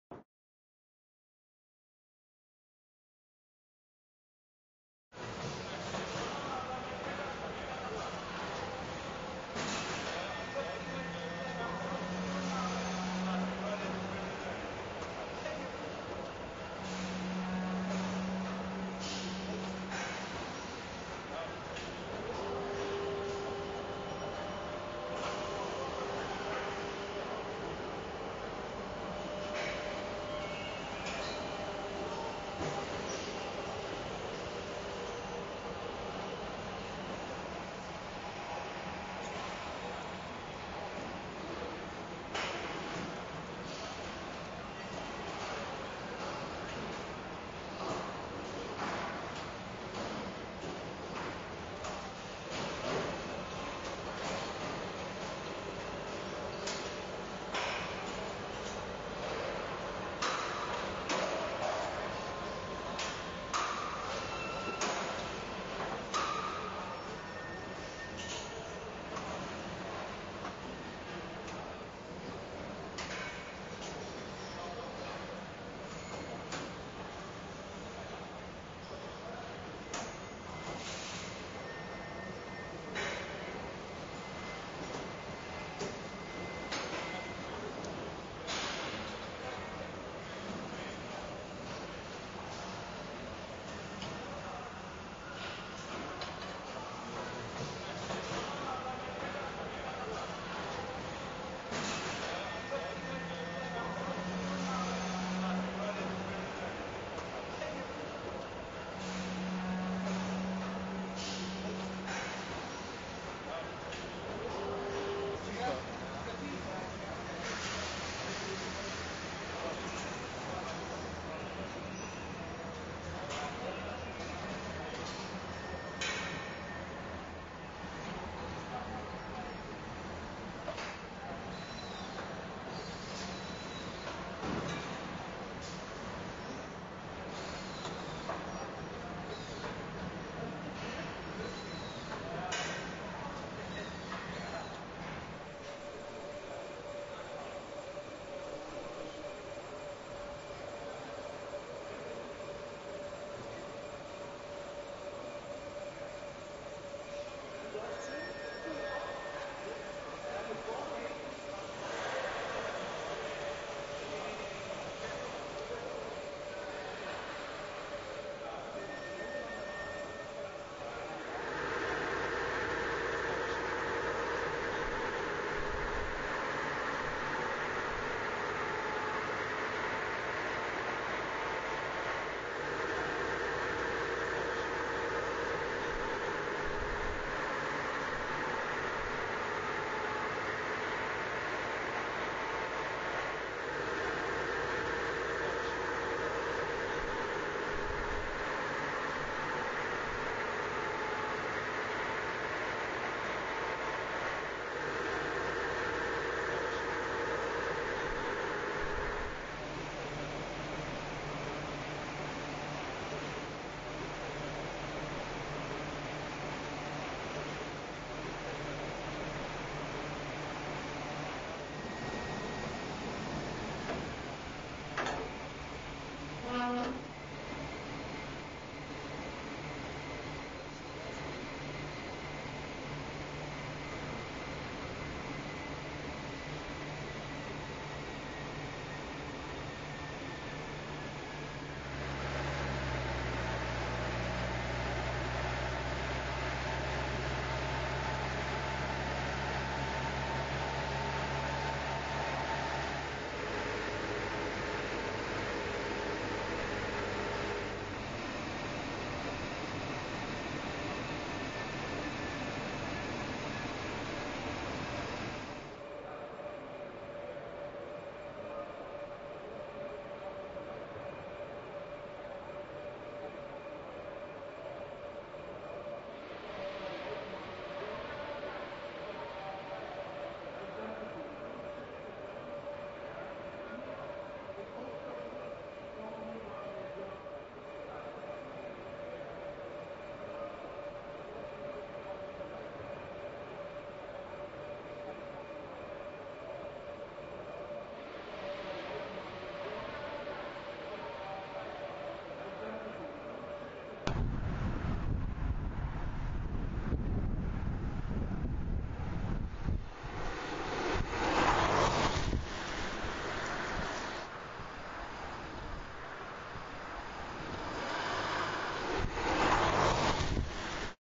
Volkswagen Scirocco
The designers of the third generation of the Scirocco unmistakably designed it to be a pure sports car. Different than the original Scirocco of the 1970s, and the second generation introduced in th...